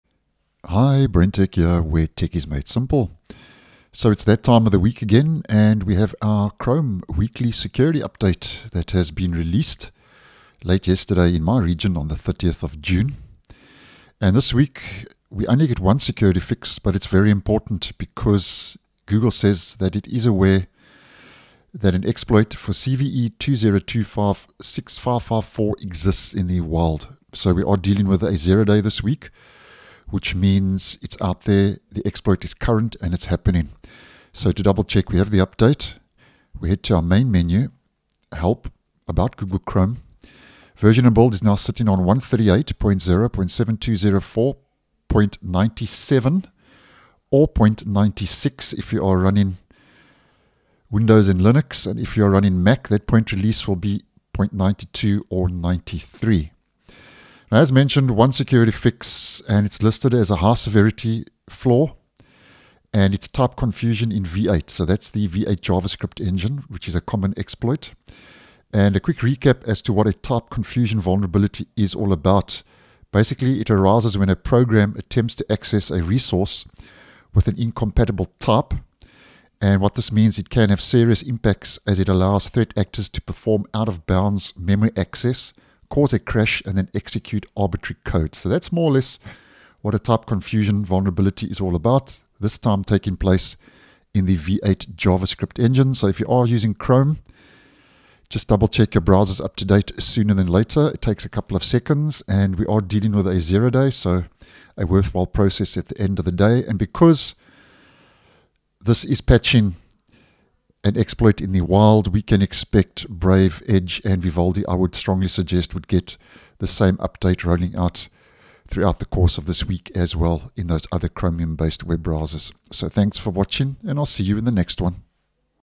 IMPORTANT: Update Chrome Now to Fix a Zero-Day Security Vulnerability!
Google is aware that an exploit for CVE-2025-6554 exists in the wild.